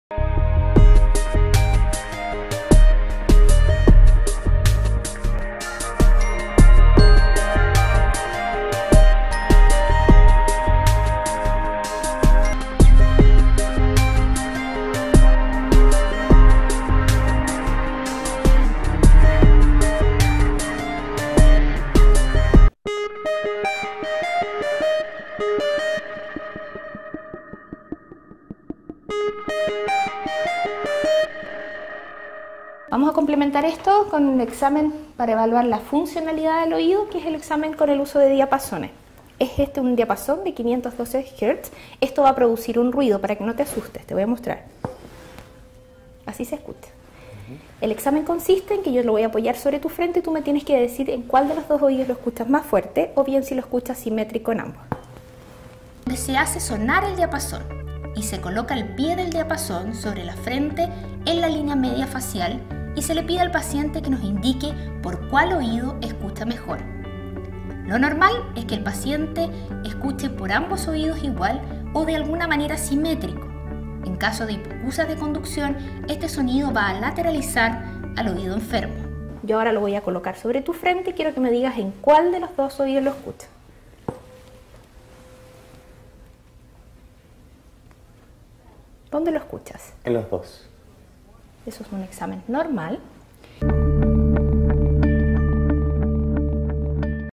🌟EXAMEN DE DIAPASÓN | TEST DE WEBER🌟
Tutor:
Marcela Castillo Franzoy.
Alumno Ayudante
Matías Jacobsen Collado.
Cristóbal Jacobsen Collado.

La prueba de Weber es una de las varios test que conforman la acumetría o prueba audiológica con diapasones. Dentro de la acumetría, los dos estudios más importantes son la prueba de RINNE y la de WEBER. Ambas son muy sencillas de realizar y además son test que nos ayudan a determinar el tipo de pérdida auditiva del paciente, de una manera OBJETIVA y desde un punto de vista cualitativo.
Cabe destacar, que el punto diferenciador de la prueba de Weber es su objetividad.

El test de Weber es de utilidad en los casos de hipoacusias unilaterales o bilaterales asimétricas, permitiendo comparar las vías óseas de ambos oídos. El procedimiento para realizar dicho estudio es la colocación del diapasón en la línea media de la cabeza o frente del paciente. Se hace vibrar el diapasón y se le debe preguntar al paciente por dónde escucha el sonido.

Test de Weber:

Weber no lateraliza: El paciente escucha centrado
Weber lateraliza al oído derecho: El paciente escucha el sonido en el oído derecho.
Weber lateraliza al oído izquierdo: El paciente escucha el sonido en el oído izquierdo.

Resultados del Test de Weber:

En personas con audición normal o con hipoacusia del mismo origen y simétricas el Test de Weber no lateraliza.
En las hipoacusias de conducción el Test de Weber lateraliza al oído peor.
En las hipoacusias neurosensoriales el Test de Weber lateraliza al oído mejor.
Mientras todos los audiólogos (audioprotesistas) realizamos la audiometría como ‘madre de todas las pruebas’ para la valoración de la hipoacusia del paciente, esta no deja de ser una prueba SUBJETIVA pero que es necesaria para cuantificar el grado de la pérdida auditiva.

Por ello, la prueba de Weber, con su objetividad tan particular, debería ser una prueba de obligado cumplimiento por el audioprotesista pero que por el contrario es poco ejecutada.